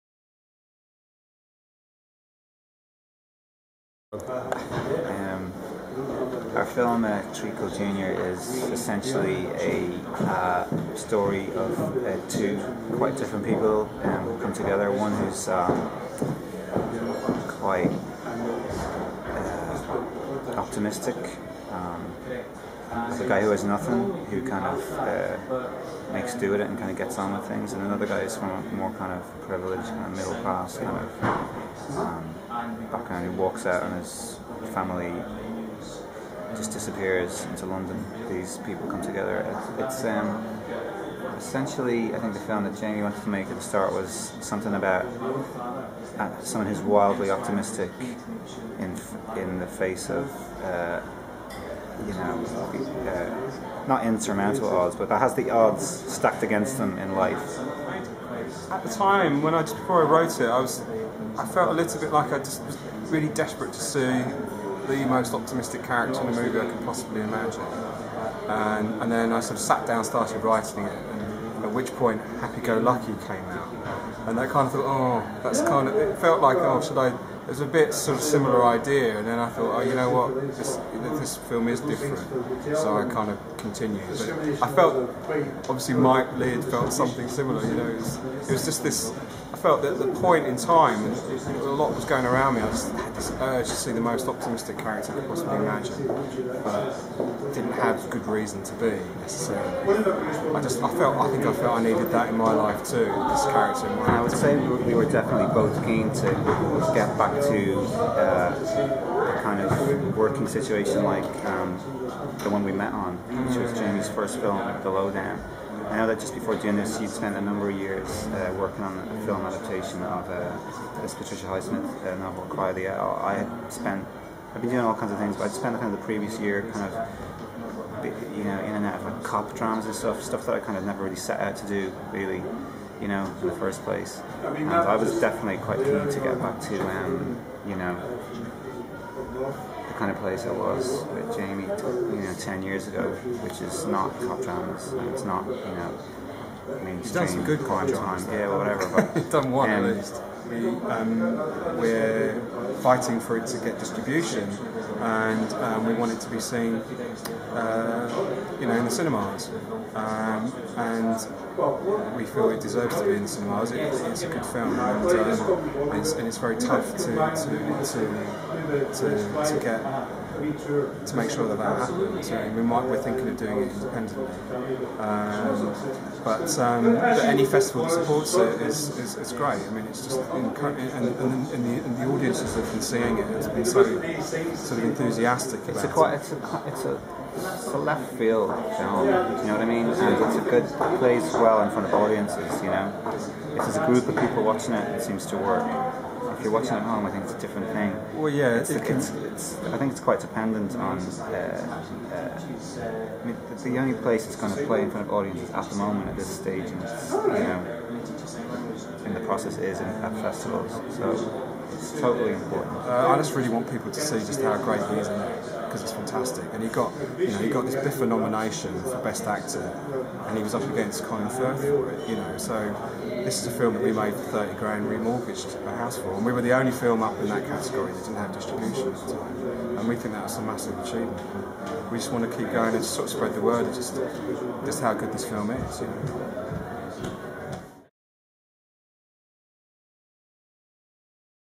Glasgow Film Festival 2011: Aidan Gillen and Jamie Thraves on Treacle Jr
Actor Aidan Gillen and writer/director Jamie Thraves visited the Glasgow Film Festival to introduce audiences to their latest collaboration, Treacle Jr.

The film salutes an unconventional friendship, beginning when depressed, middle-class Tom (Tom Fisher) walks out on his wife and baby and heads to London.

He is attacked whilst sleeping rough and regains consciousness in casualty where he is befriended by the affable, irrepressible, endearingly child-like Aidan (Aidan Gillen).

Gillen and Thraves talked to Festival vlogger, Jonathan Melville, about their hopes for Treacle Jr and the importance of taking it to film festivals such as Glasgow.